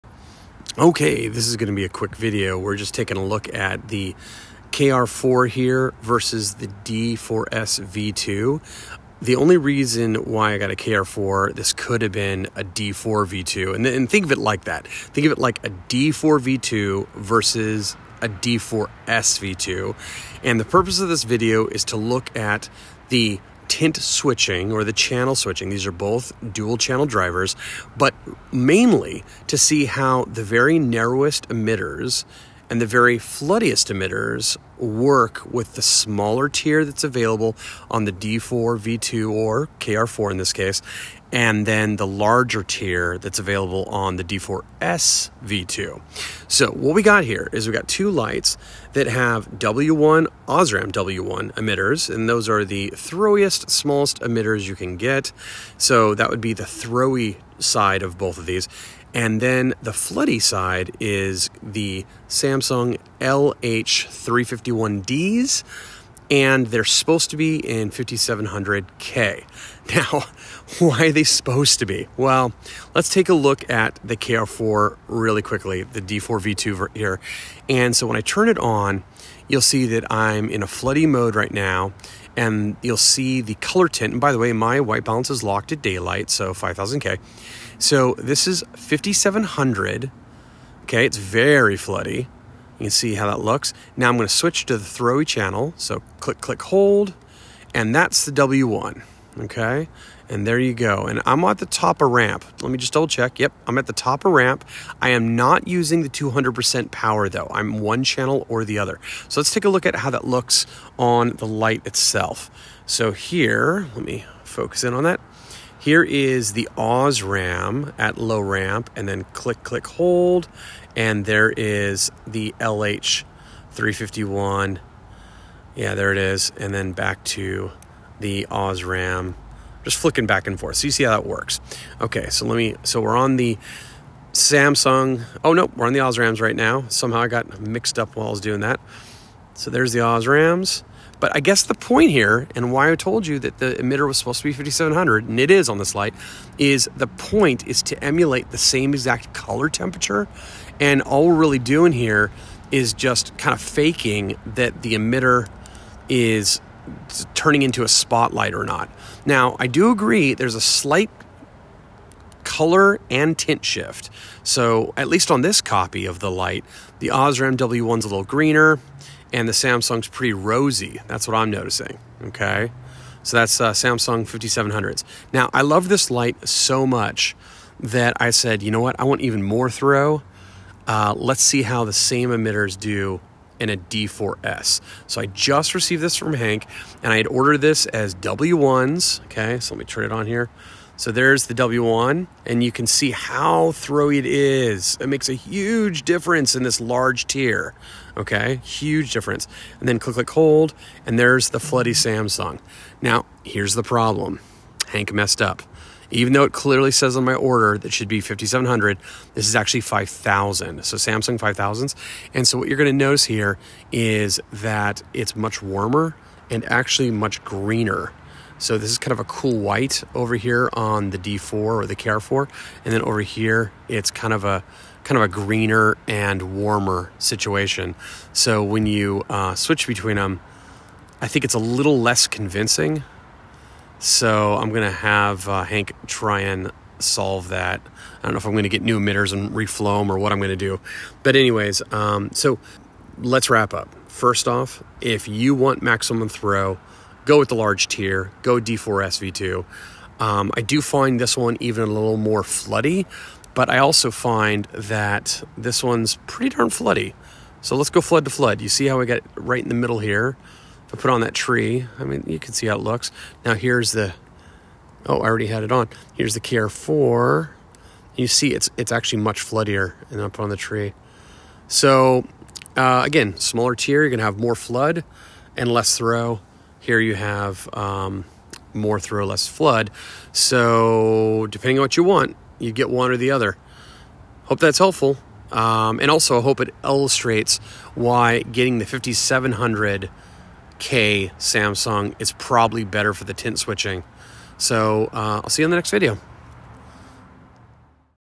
KR4 (D4V2) vs D4SV2 instant channel switching (Throw and Flood)
Let's compare two dual channel lights set up to flood and throw.  Both of these lights have OSRAM W1 for throw and Samsung LH351D for flood.  But how is the TIR optic differences between the Emisar KR4 (D4V2) and D4SV2 going to affect the same emitters?

Oh, and the D4SV2 has the new 26800 battery tube, and a QB26800 (Queen Battery from Aloft Hobbies

You can purchase these flashlights from Hank Wang directly

Important Edit: Hank and I got to the bottom of what was going on with the LH351D emitters.  They were in fact 5700K.  The CCT difference between the KR4 and the D4SV2 has to do with the optics.

Contents:
00:00 - intro
01:12 - KR4 / D4V2 beam shots
01:52 - What Instant Switching Looks Like
03:00 - D4SV2 Beam Shots
04:03 - Recommendations